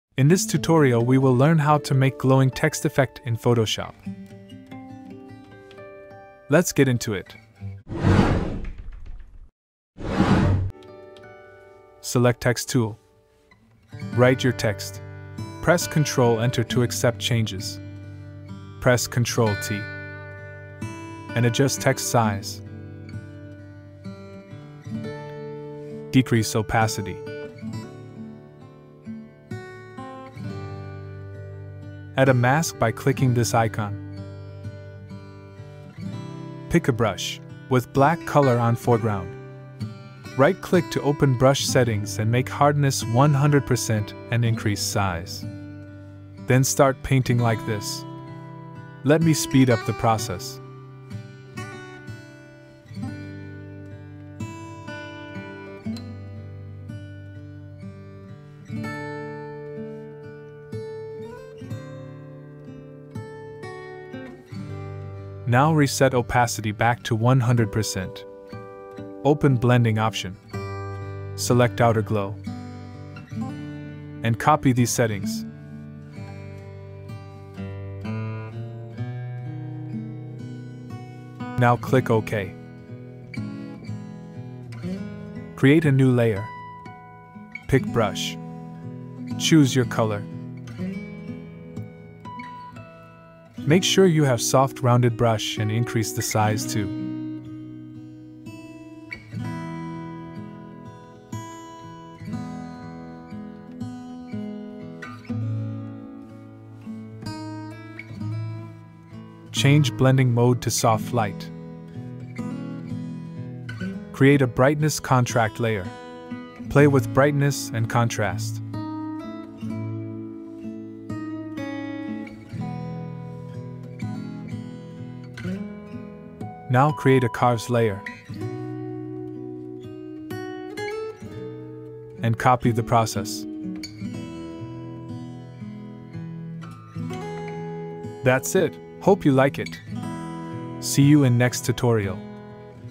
Glowing Text Effect Tutorial in Adobe Photoshop
How to Create Glowing Text Typography Effect in Adobe Photoshop